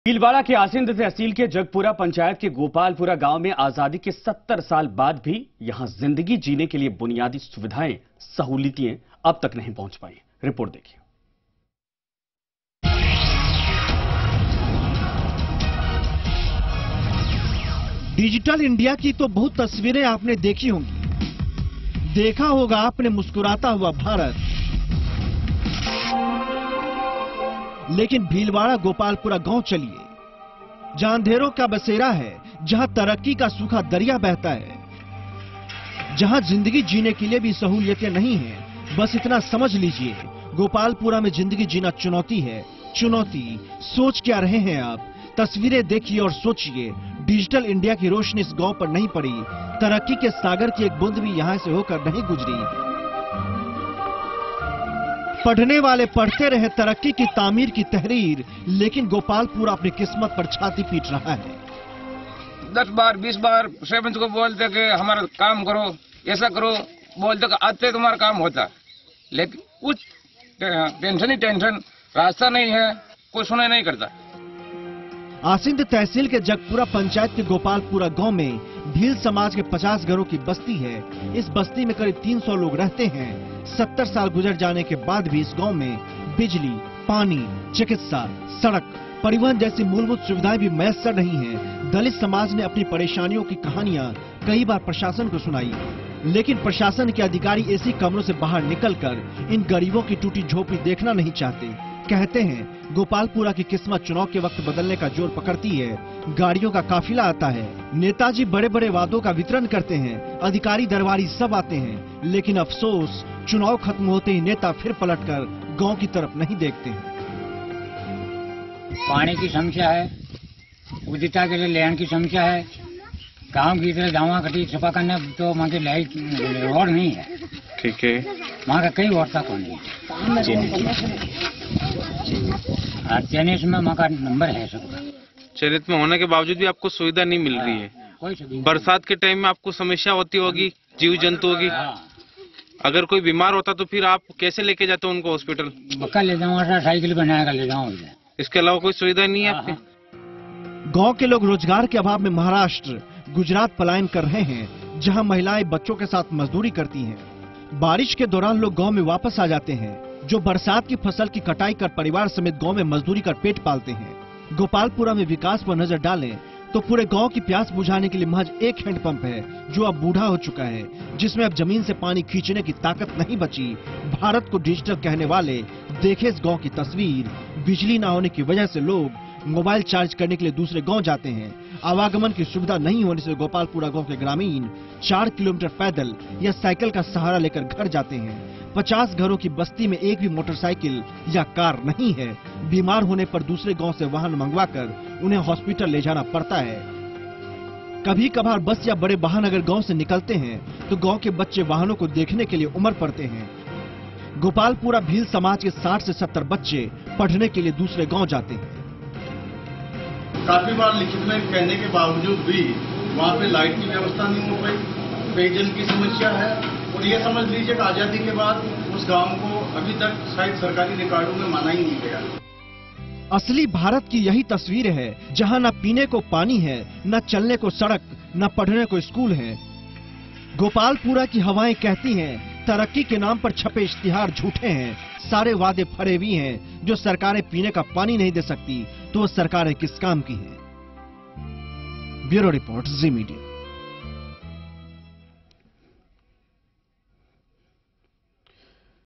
अंधेरे में डूबा गांव
डिजिटल इंडिया की तो तस्वीरें आपने बहुत देखी होंगी...देखा होगा आपने मुस्कराता हुआ भारत...लेकिन भीलवाड़ा गोपालपुरा गांव चलिए...जहां अंधेरों की बसेरा है...जहां तरक्की का सूखा दरिया बहता है..